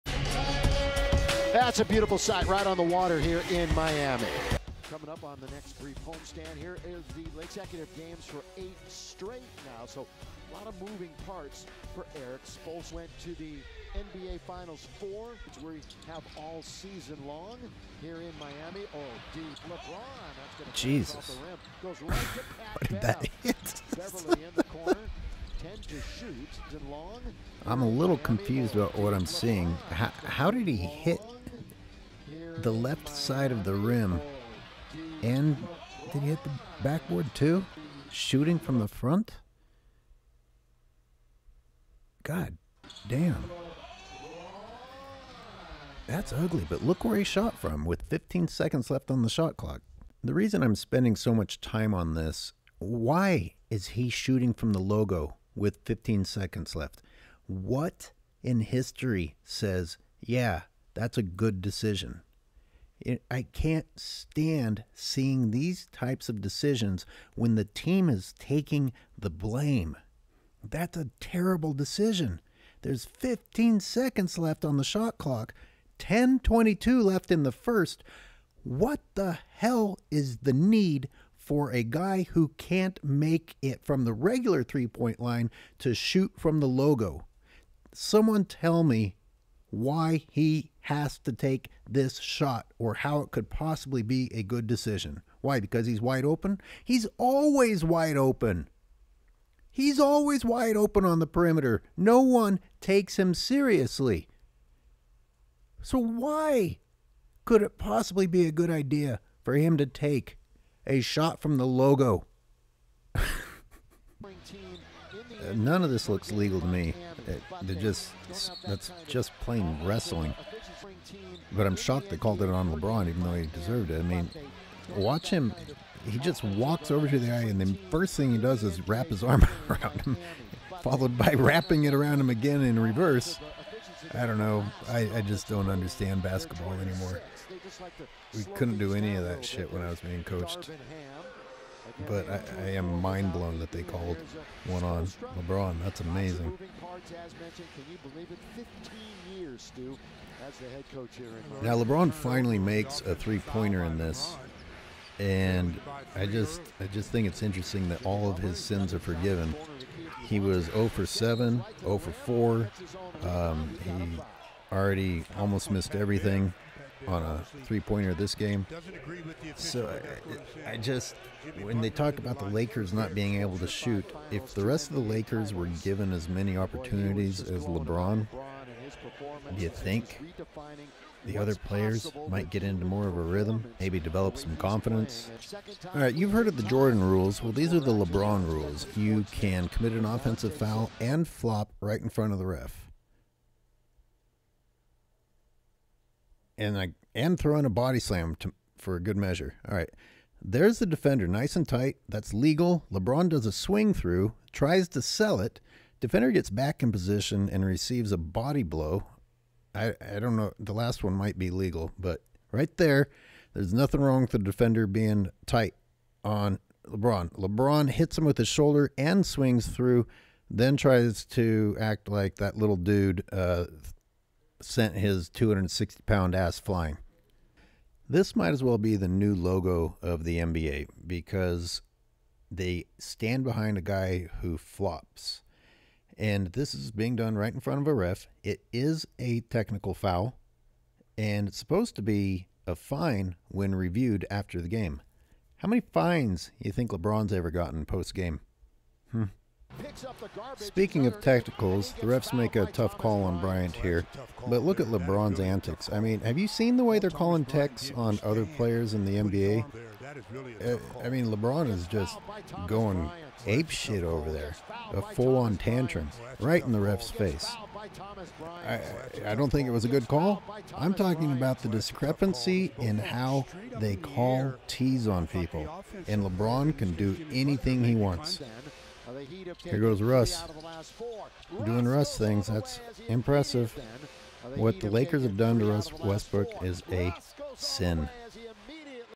Lakers Heat 12-28-22 Q1
Los Angeles Lakers fall to the Miami Heat on 12-28-22.